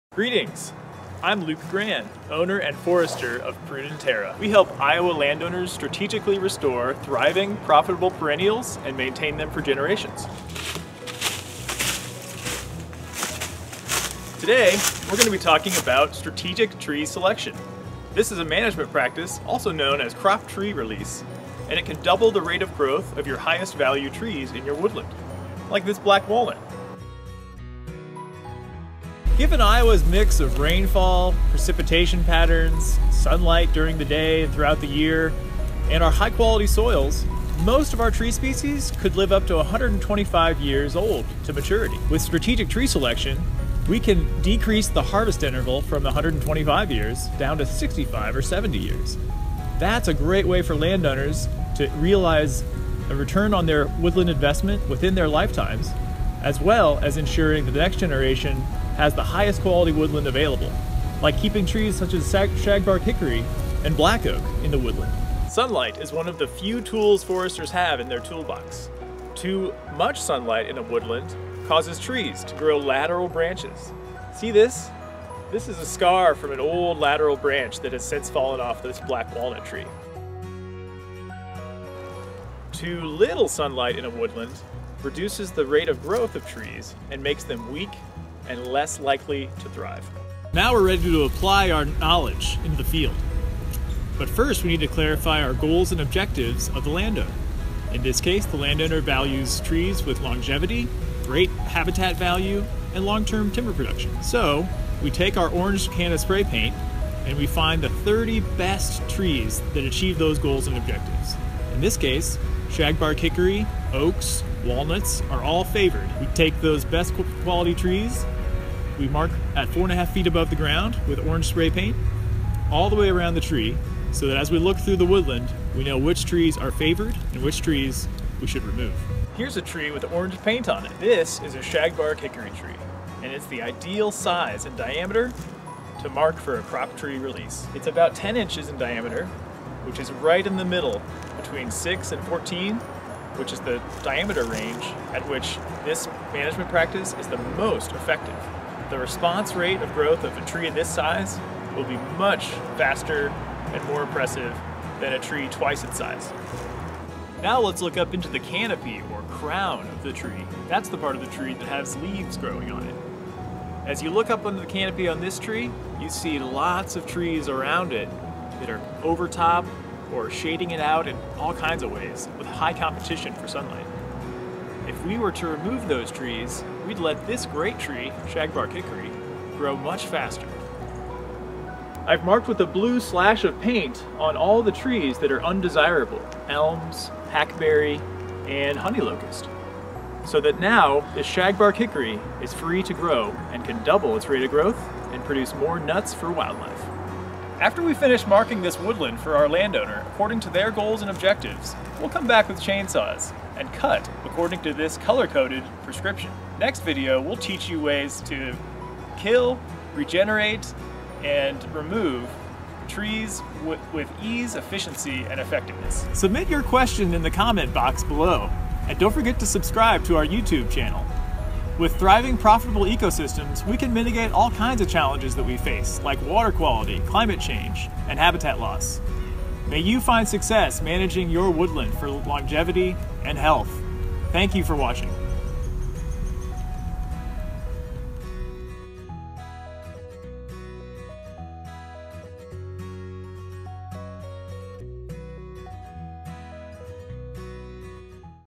Strategic Tree Selection
Learn how to maximize wildlife habitat and timber production in your woodland. In this video, forester Luke Gran will teach you how to strategically remove poorly formed and low value trees so that trees of high wildlife and timber value can thrive.

Strategic Tree Selection is based on a common forestry practice called Crop Tree Release. See this publication by Perkey and Wilkins for details:

Find out more about Prudenterra's forestry work at prudenterra.com.